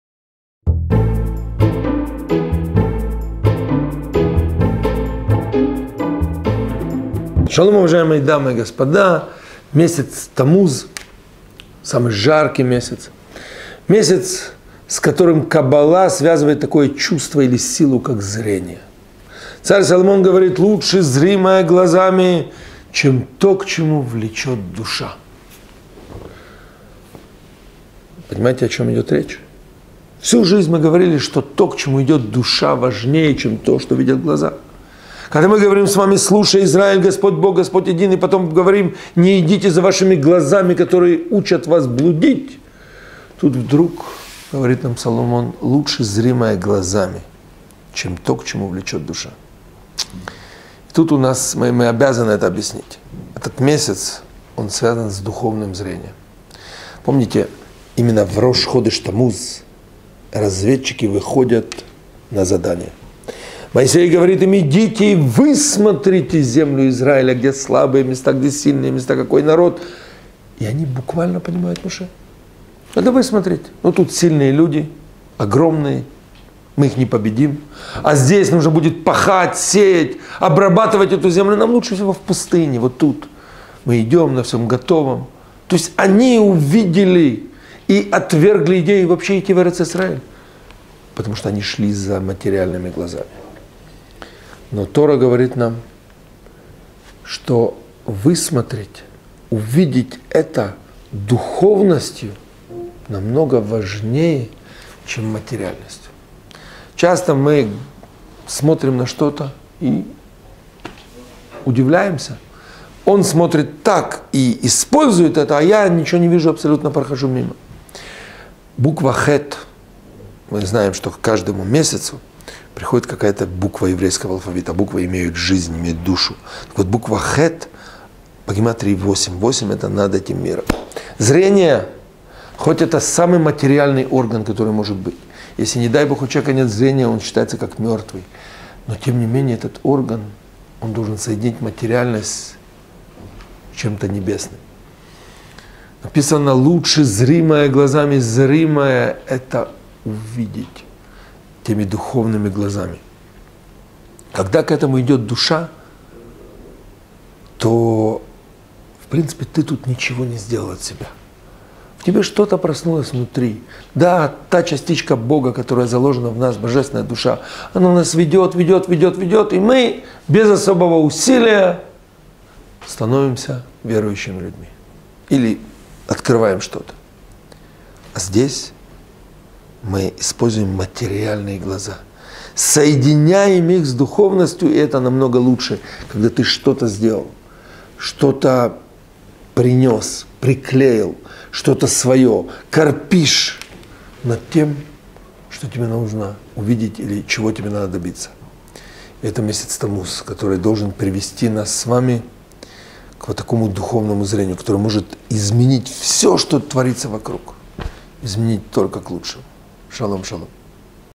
🏆 Путь к успеху. Сила зрения | Рав Алекс Артовский
Шалом, уважаемые дамы и господа!
Наступил месяц Тамуз - самый жаркий месяц. Месяц, с которым Каббала связывает такую чувство и силу, как зрение.

Царь Соломон говорит: "Лучше зримое глазами, чем то, к чему влечет душа".
Вы понимаете, о чем идет речь? Всю жизнь мы говорили, что то, к чему идет душа, важнее, чем то, к чему идет глаза...